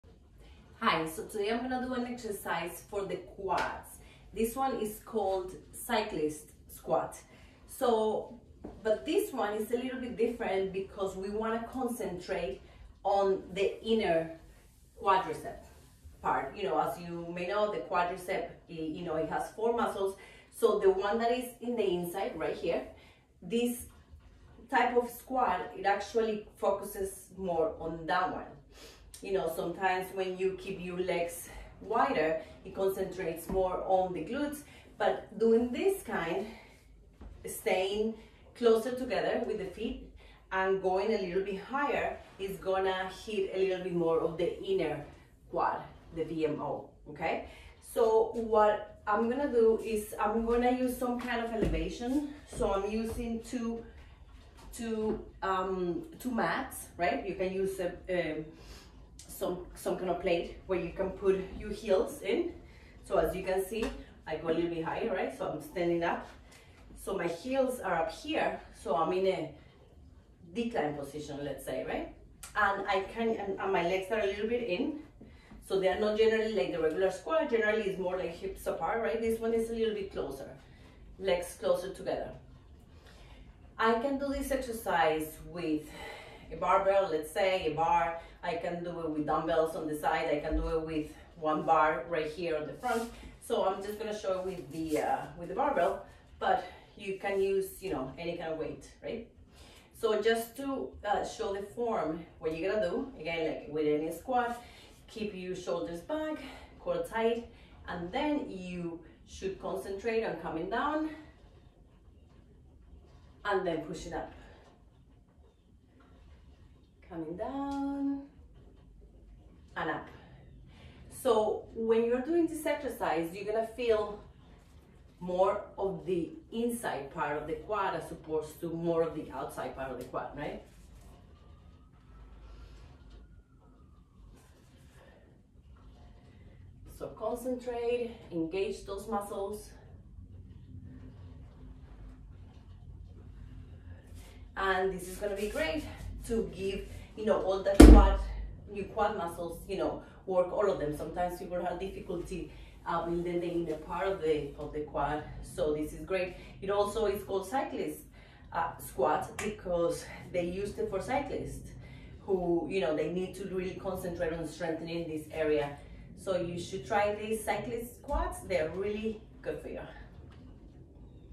Cyclist Squat
This video shows how to do Cyclist Squats. The Cyclist Squat is a leg exercise that strengthens the vastus medialis, the ‘teardrop’ portion of the quad muscles.

To a Fitter Healthier You,

ADRIANA ALBRITTON
The Fitness Wellness Mentor